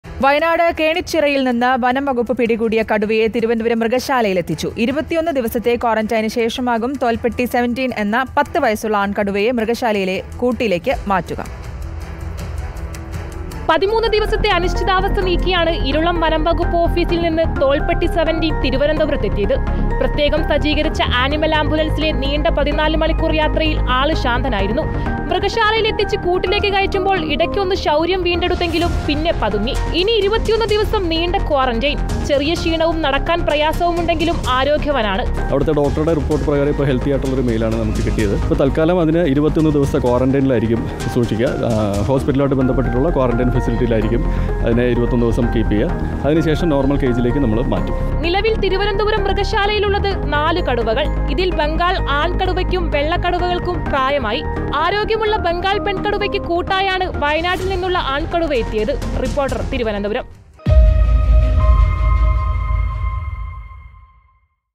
'ബം​ഗാൾ പെൺ കടുവയ്ക്ക് കൂട്ട് തോൽപ്പെട്ടി 17' | Thiruvananthapuram Zoological Park | Kenichira Tiger
കേണിച്ചിറയില്‍ നിന്നും തലസ്ഥാനത്ത്; 'ബം​ഗാൾ പെൺ കടുവയ്ക്ക് കൂട്ട് തോൽപ്പെട്ടി 17' | Thiruvananthapuram Zoological Park | Kenichira tiger

With Regards
Team RBC